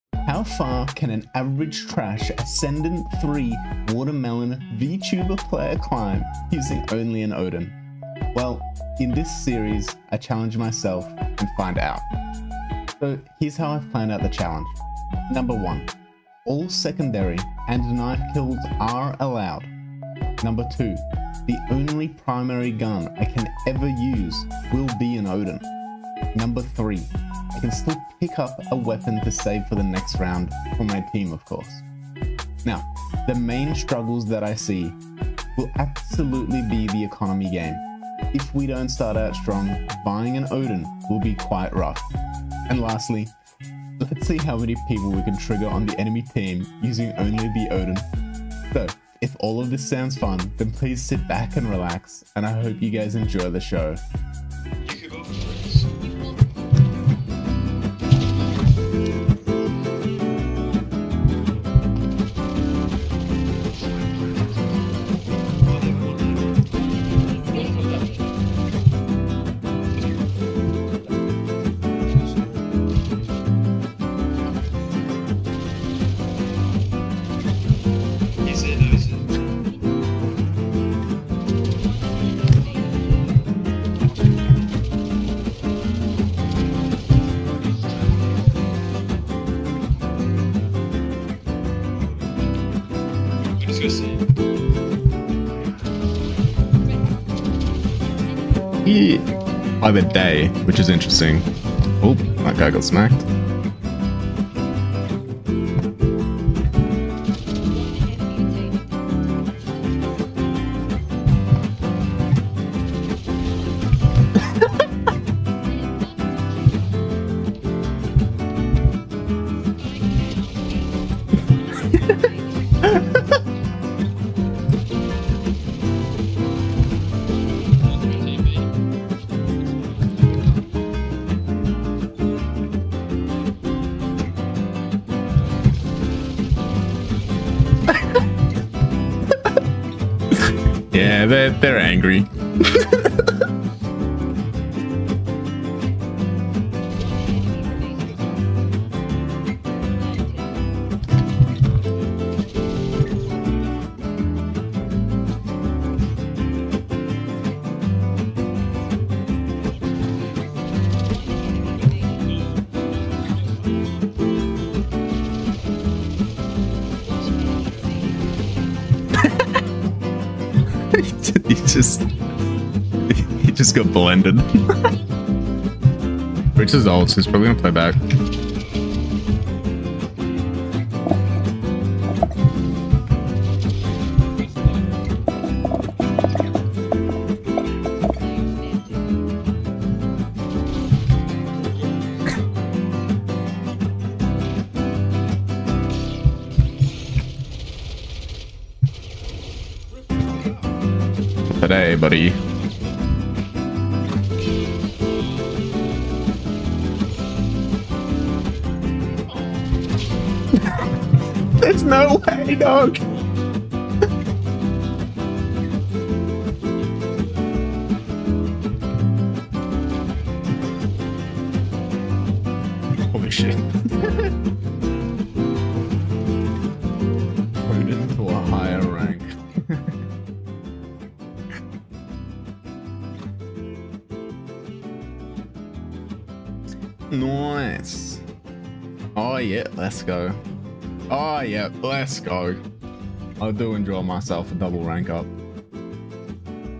ODIN To Immortal Begins... I placed in....
Can an Ascendant 3 player make it to Immortal using just the ODIN? Well, I decided to find out by starting a fresh account and climbing the ranks all over again in this ODIN to Immortal challenge.